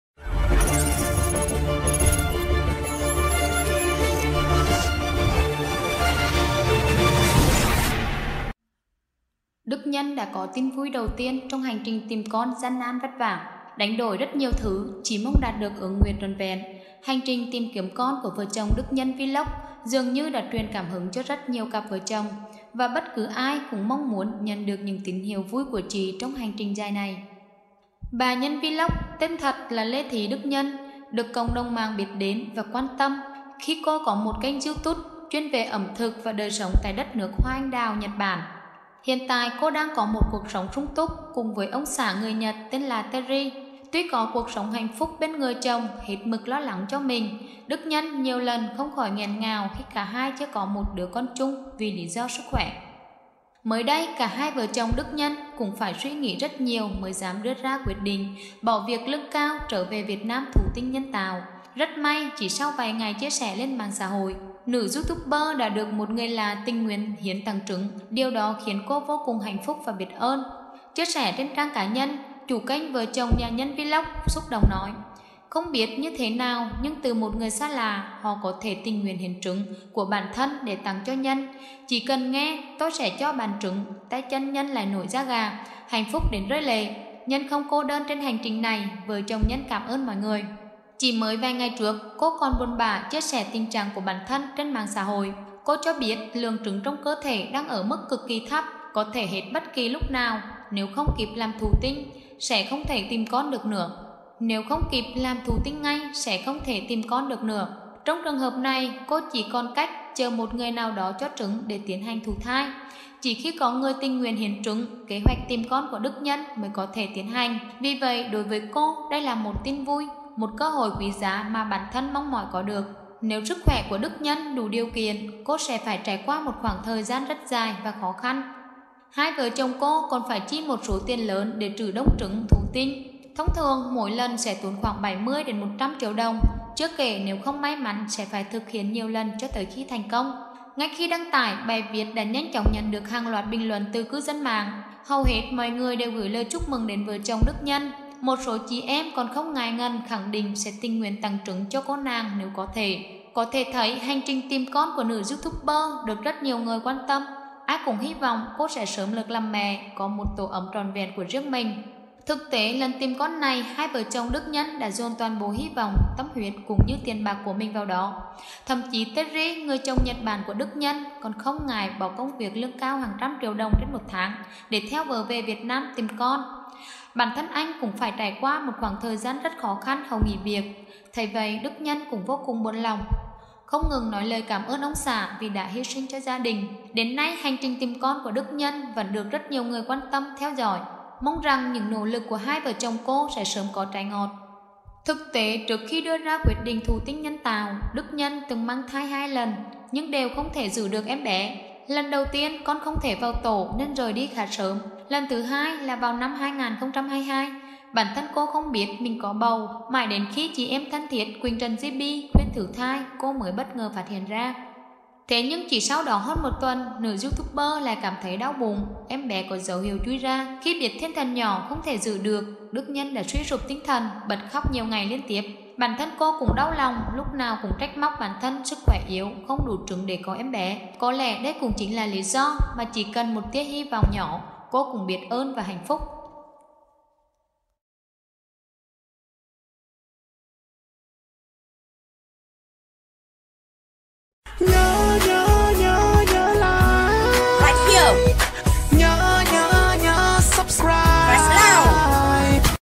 Đức Nhân Vlog đã có TIN VUI đầu tiên trong hành trình “tìm con” gian nan vất vả. CDM chúc mừng
▶ Tin 24h là kênh tổng hợp các video về tình yêu cuộc sống, hôn nhân gia đình...